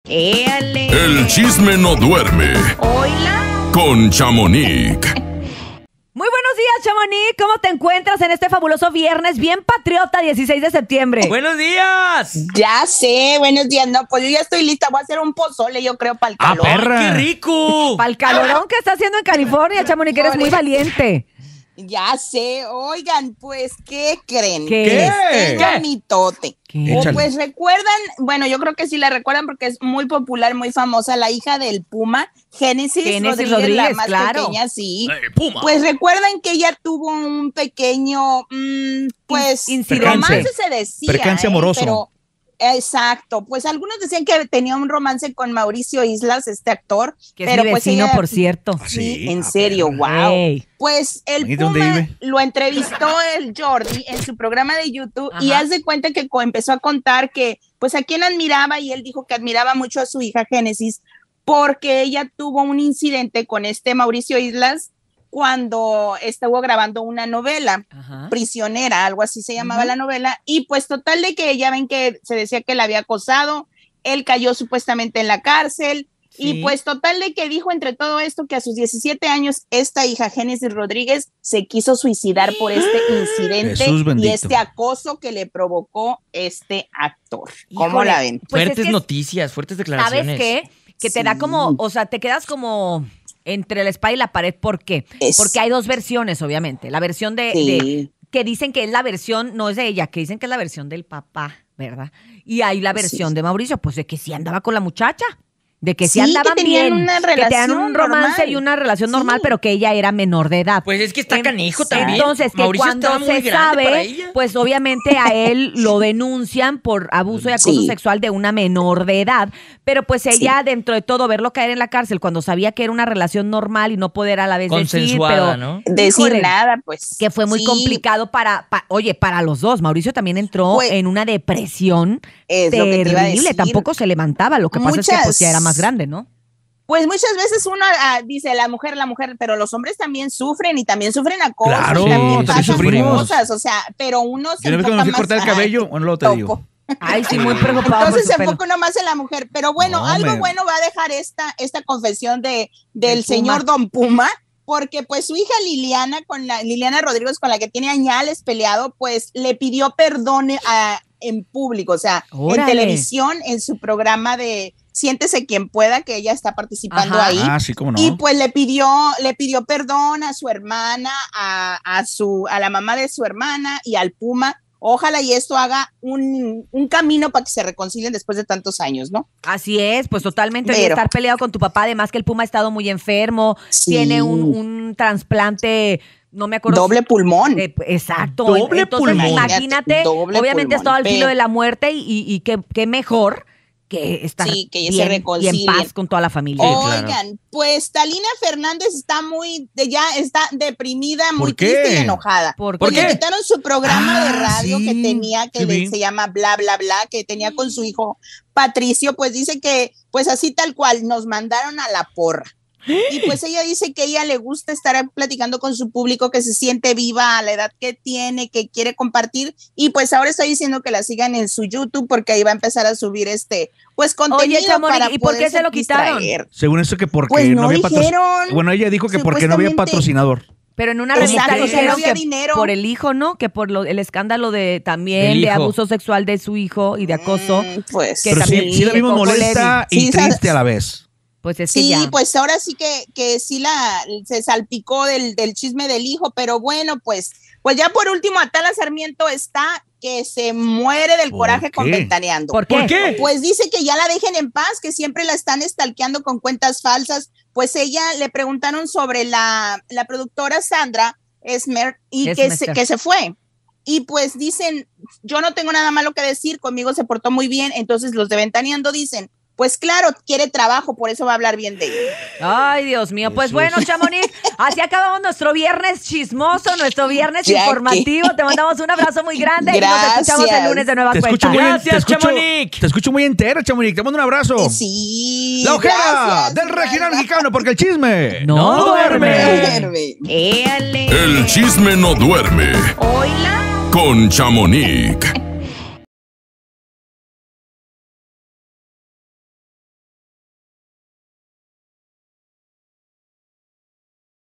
Atala Sarmiento acusa que en Ventaneando la stalkean con cuentas falsas
El chisme no duerme con Chamonic: El puma revela que a la persona que admira es a su hija Génesis por esta razón. Además, la ex conductora de TV Azteca Atala Sarmiento arremete contra colaborador de Ventaneando que la llamó "urgida de empleo"